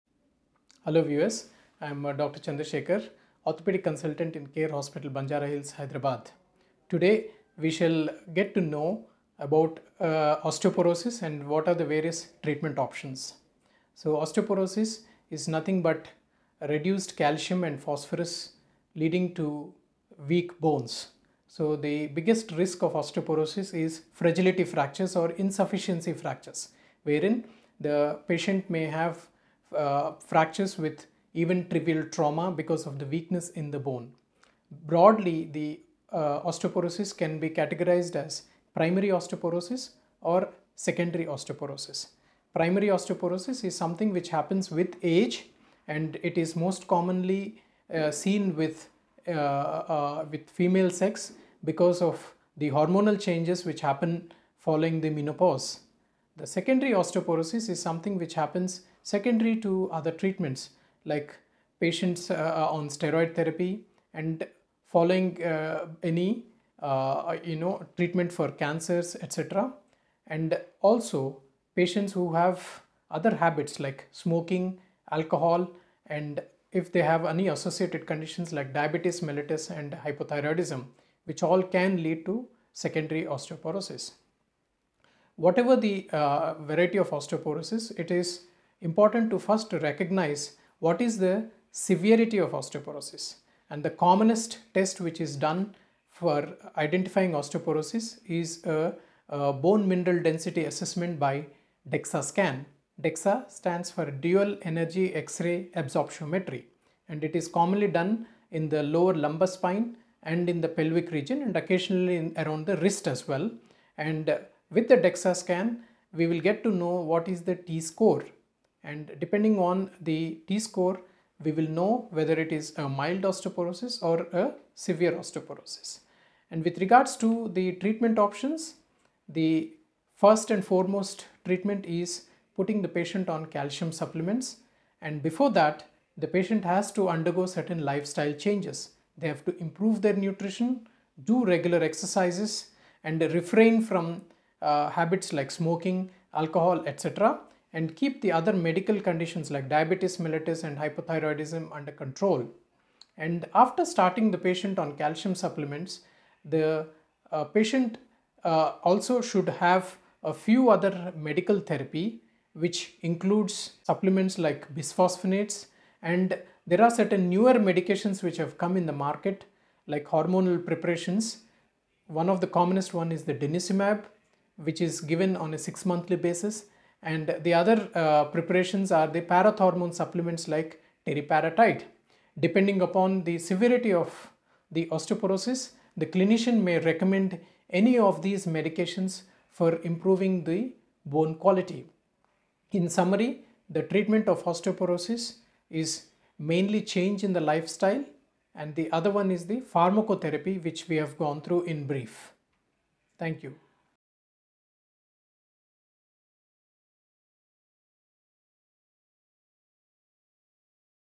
Osteoporosis: Types, Causes, Symptoms & Treatment | Dr. Chandra Sekhar Dannana | CARE Hospitals
If you're concerned about your bone health, watch this video to learn about osteoporosis, its types, causes, symptoms and treatment. Dr. Chandra Sekhar Dannana, Senior Consultant Orthopaedics, CARE Hospitals, Banjara Hills, Hyderabad, will discuss the various treatments available and explain the risks and benefits of each.

CARE Hospitals Group is a multi-specialty healthcare provider with 16 healthcare facilities serving 8 cities across 6 states in India.